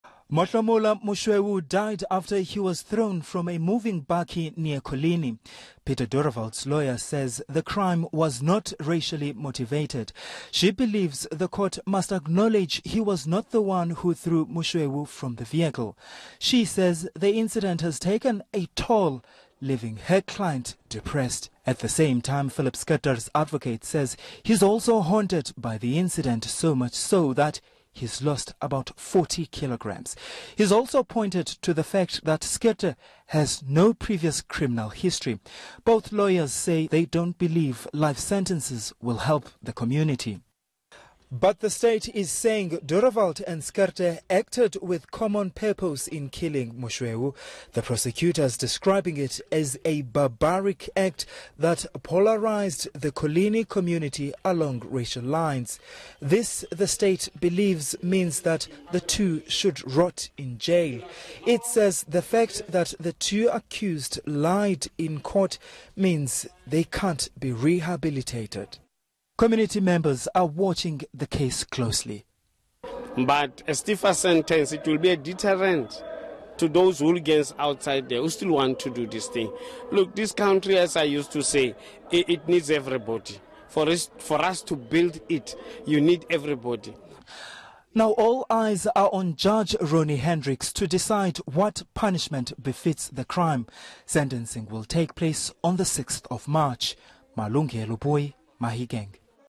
The lawyers for two convicted killers say they don’t deserve a life sentence
The men killed teenager Motlhomola Moshoeu, after he allegedly stole a sunflower.
Their legal team is now asking for mercy, saying their clients have also suffered.
eNCA's Malungelo Booi reports. Courtesy